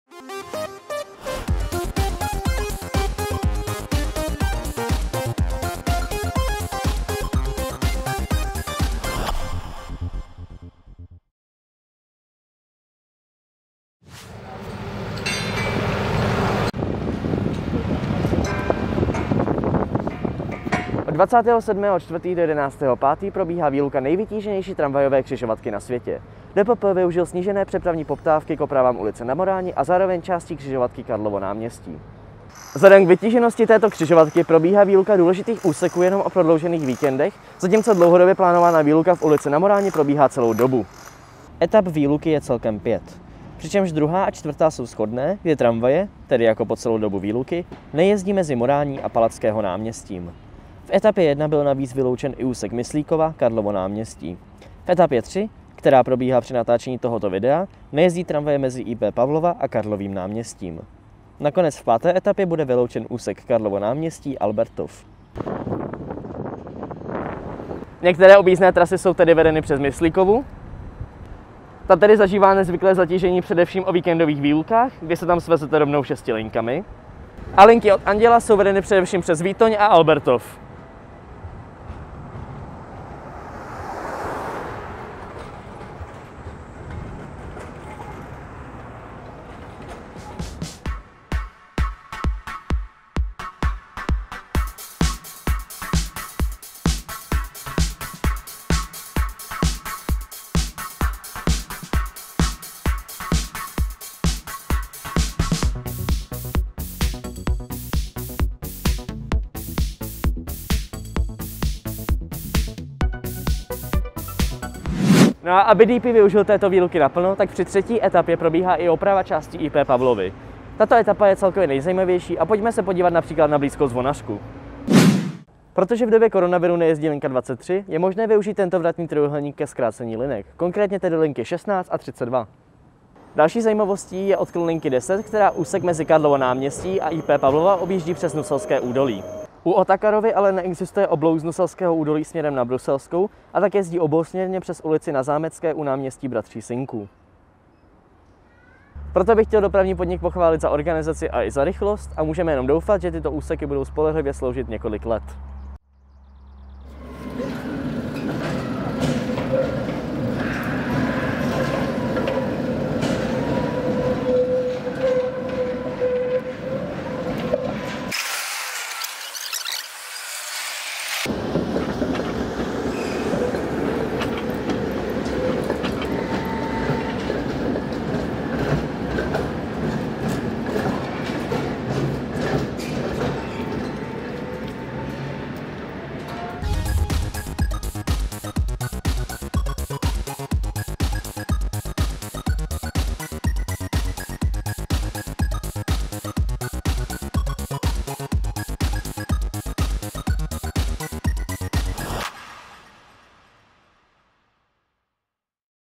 Pět etap na Karlově náměstí | VÝLUKA
Omezení provozu umožnilo Dopravnímu podniku spustit a rychle provést sérii krátkých výluk na nejvytíženější křižovatce Karlovo náměstí. Během pěti etap byly rekonstruovány v podstatě všechny větve.

Pokud se ti video líbilo, můžeš ho sdílet ;)